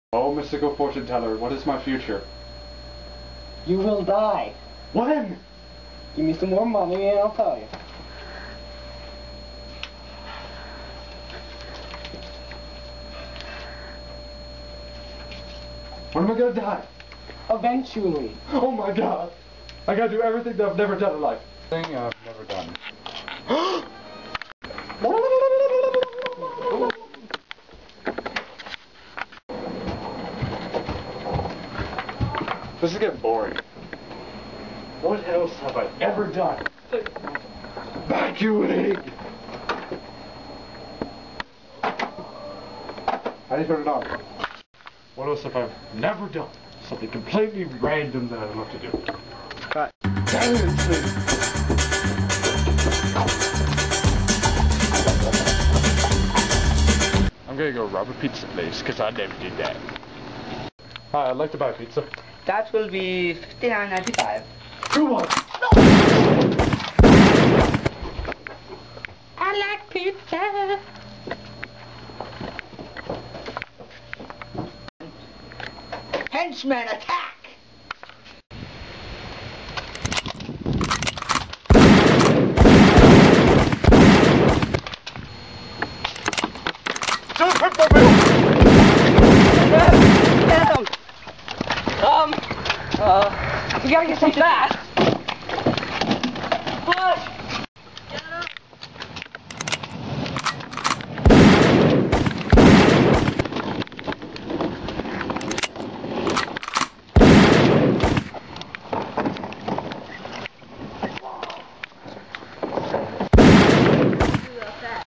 SchwaCo- Jimmy's Last Stand
This is what happens when Jimmy finds out that everyone dies at some time in there lives.